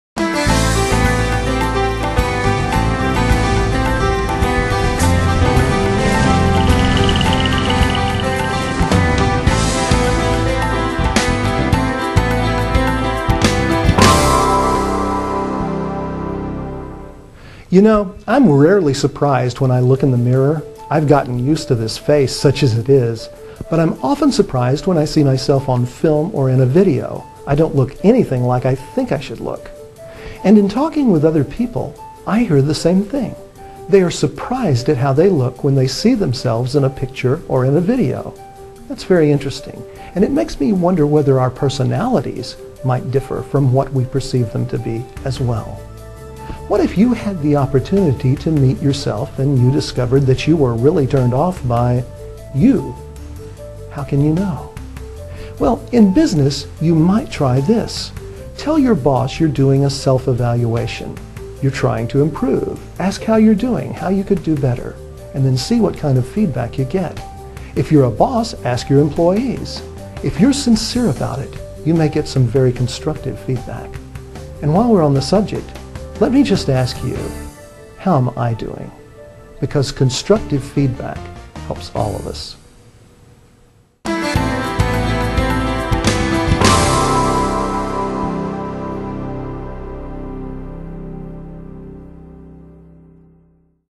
JEFF NEILL: "LOOKING IN THE MIRROR"
What do you see when you look in the mirror? Is it really what other people see when they look at you? How can we know what we really look like to other people? There's some great advice in this Breakout Moment.